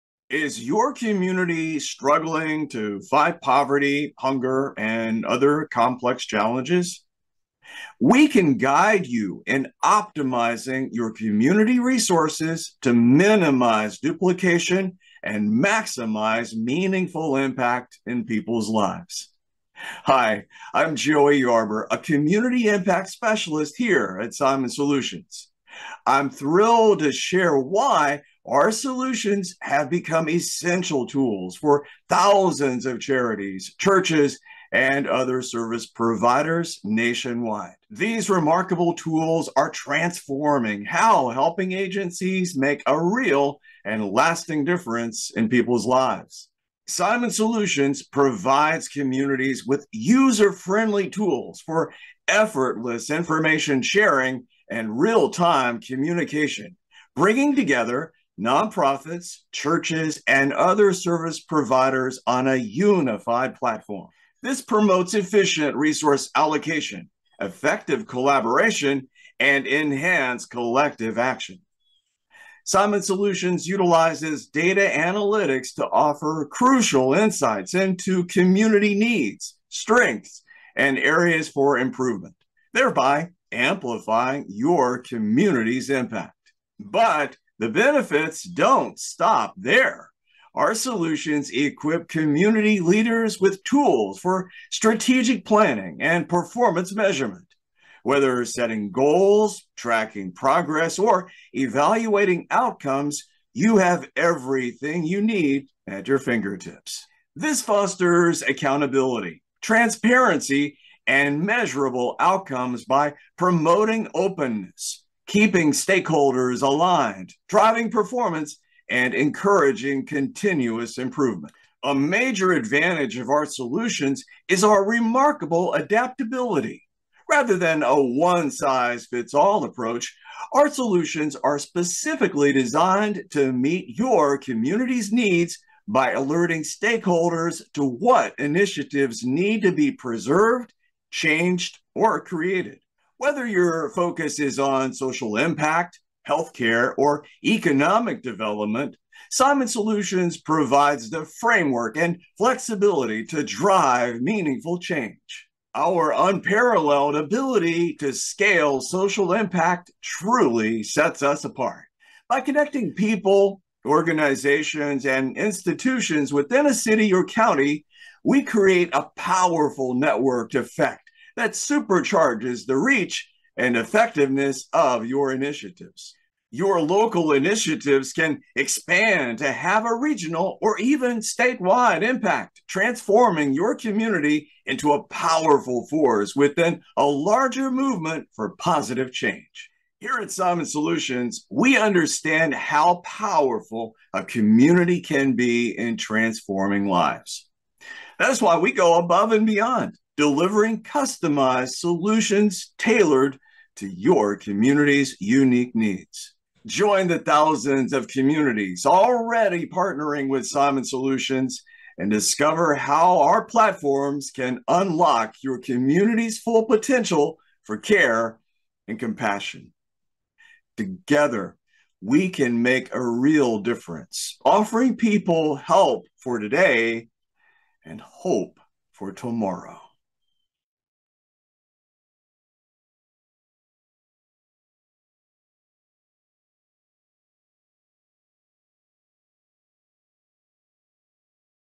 Simon Solutions. Empowering Thousands of Communities in over 49 states
At Simon Solutions we believe in harnessing the power of technology to strengthen communities. Our innovative tools and software help nonprofits, churches, and service providers streamline their operations, making it easier to connect people to vital resources and services. From CharityTracker to OasisInsight, we offer solutions that foster collaboration, improve efficiency, and drive greater community impact.

Whether you're looking to manage casework, track outcomes, or collaborate with local partners, Simon Solutions has the platform you need to make a difference. Our tools are designed to help you do more good—faster and more effectively.